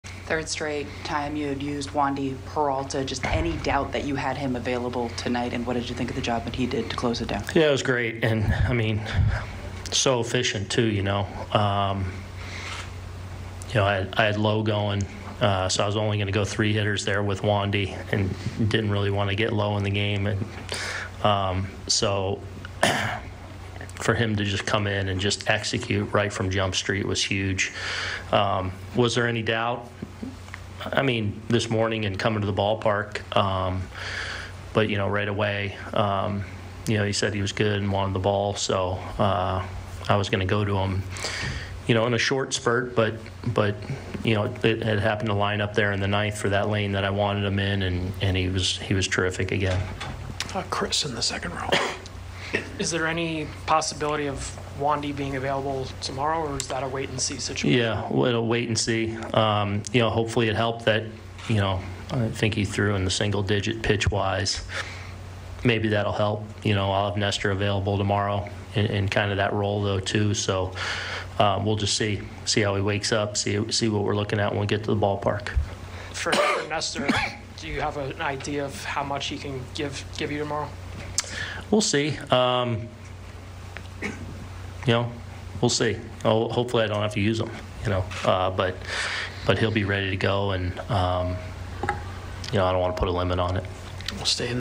Aaron Boone on closing out Game 4 with Wandy Peralta
Aaron Boone talks about using Wandy Peralta to close out Game 4 with a 4-2 victory in Cleveland.